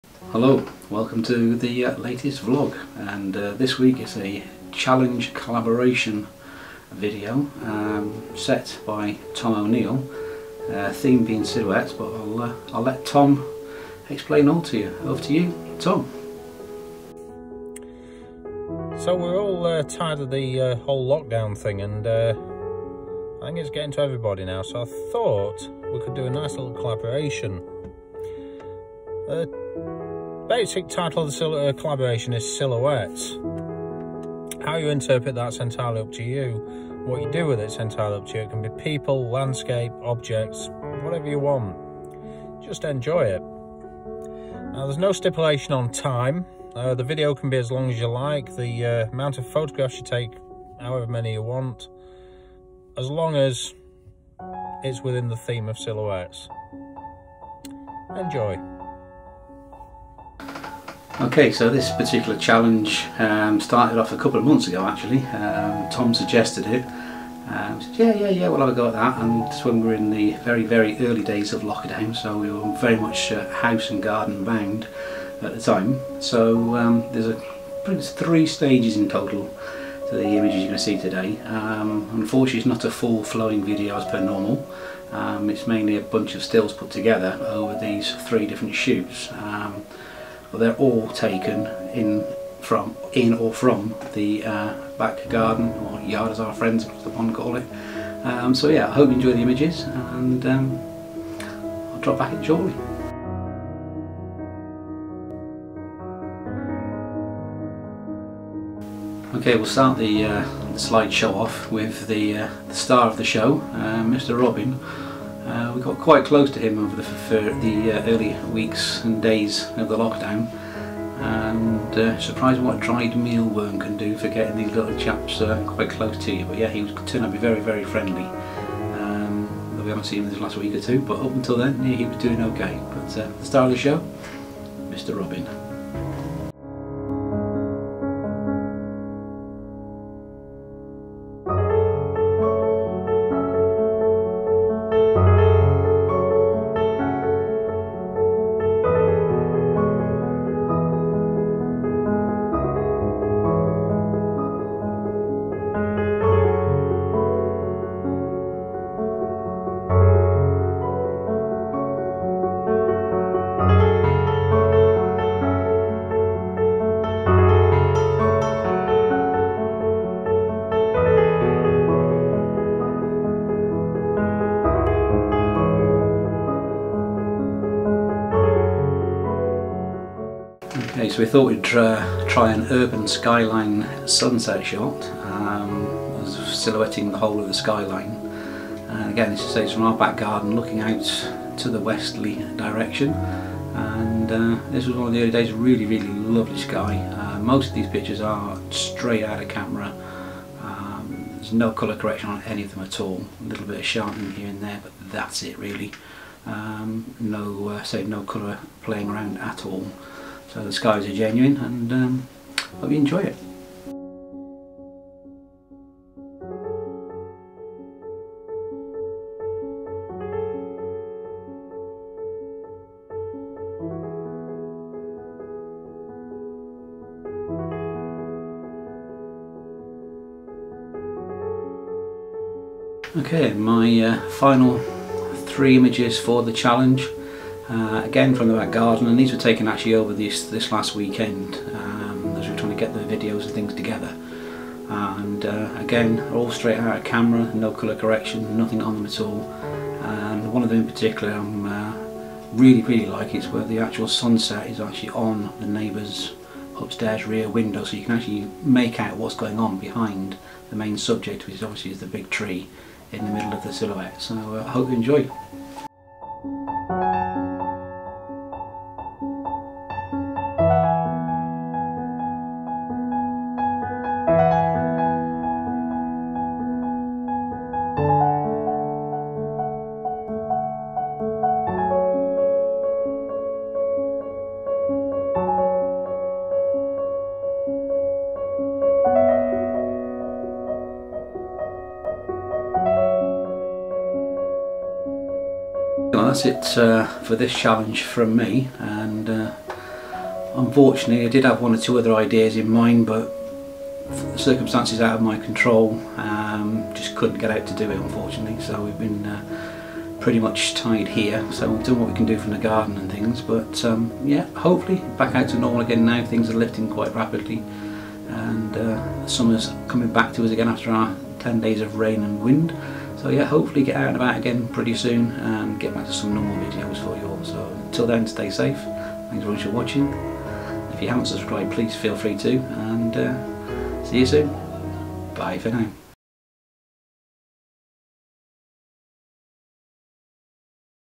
Silhouette Challenge 2020
Collaboration Challenge to capture Silhouettes, set by Tom O'Neill, hope you enjoy it, links below to the other photographers/creatives taking part.

Please show them all some support, see you soon. Mark